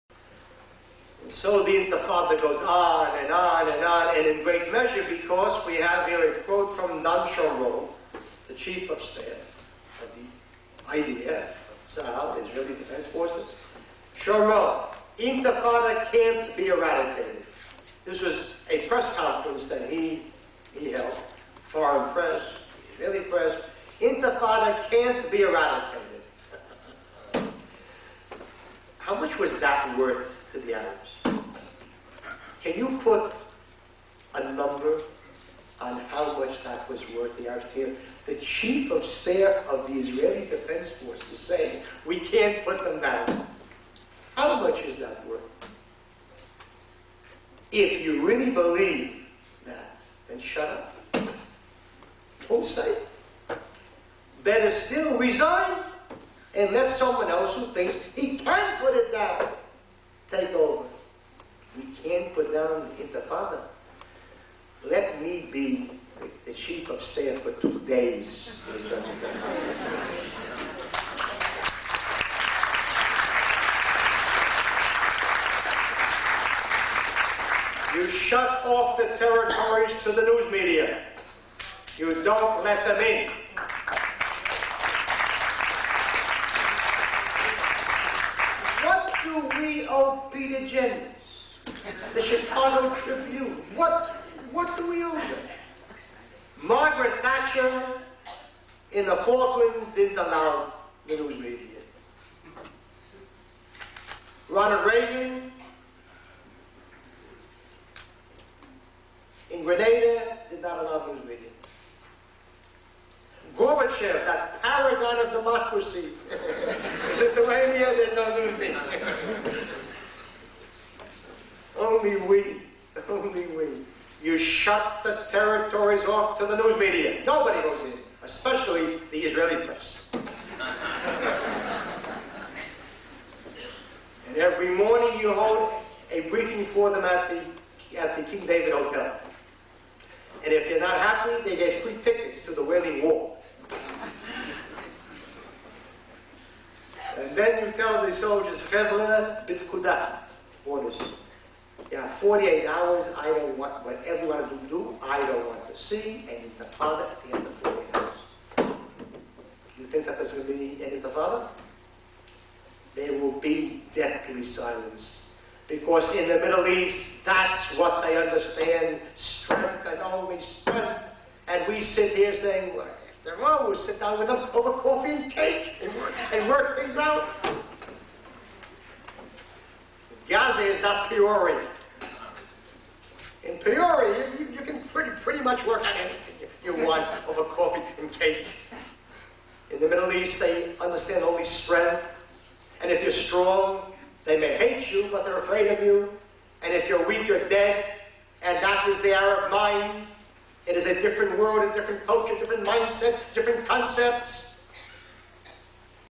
15.11 Рабби Меир Кахане הי"ד Начальник генштаба Армии Обороны Израиля заявляет
- Начальник генштаба Армии Обороны Израиля заявляет: "Пресечь интифаду невозможно". Если ты так думаешь - уйди в отставку и освободи место для того, кто может её пресечь. ... На Ближнем востоке понимают только силу. Если ты силён - тебя будут ненавидеть, но уважать. Если ты слаб - то ты труп. Таков арабский менталитет.